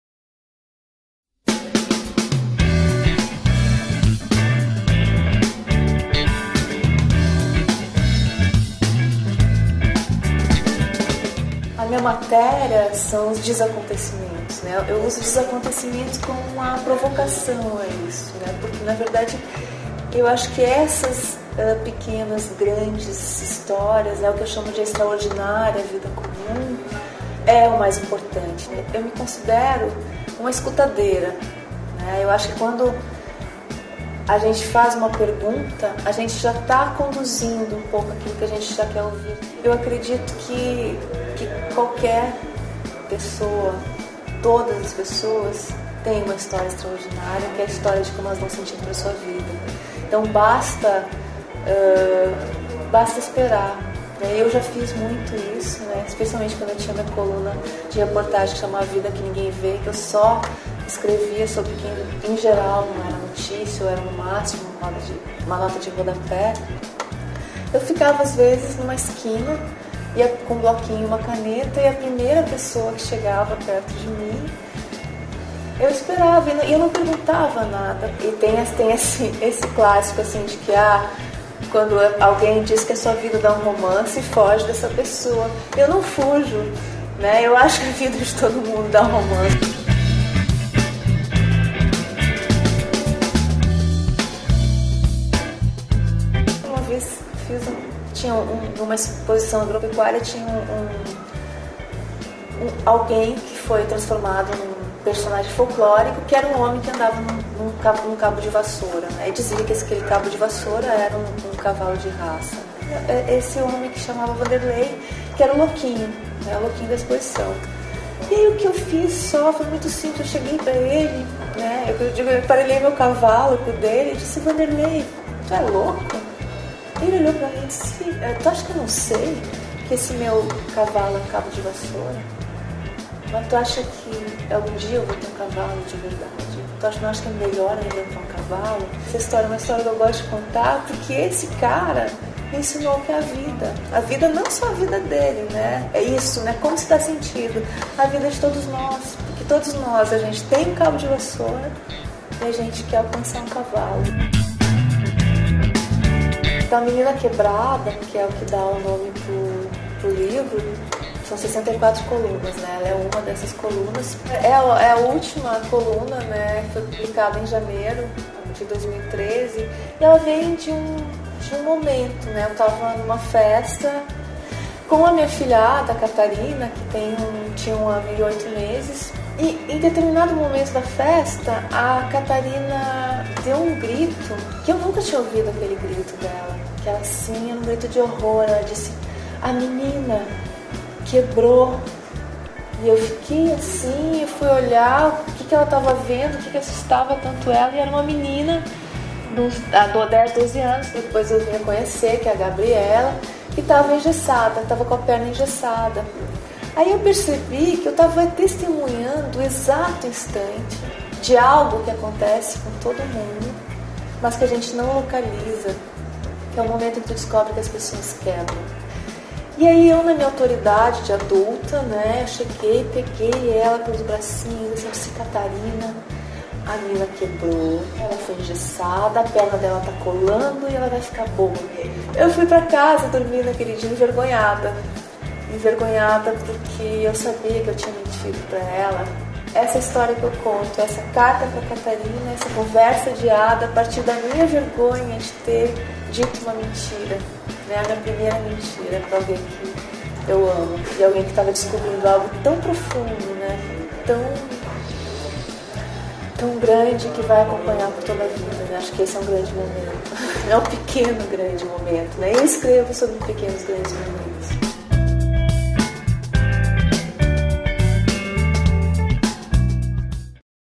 Eliane Brum e os pequenos grandes momentos cotidianos
Por Maria Fernanda Moraes

Uma jornalista de desacontecimentos. É assim que Eliane Brum se define. Ela também costuma dizer que os protagonistas de suas histórias são pessoas e não personagens. "Para não correr o risco de ir atrás de um personagem que se encaixe numa narrativa já pronta. O jornalista precisa partir de um 'não-saber'", afirma.

Neste bate-papo com o SaraivaConteúdo, Eliane fala sobre os pequenos grandes momentos que escreve e de como dá sentido à sua vida contando as histórias dos outros.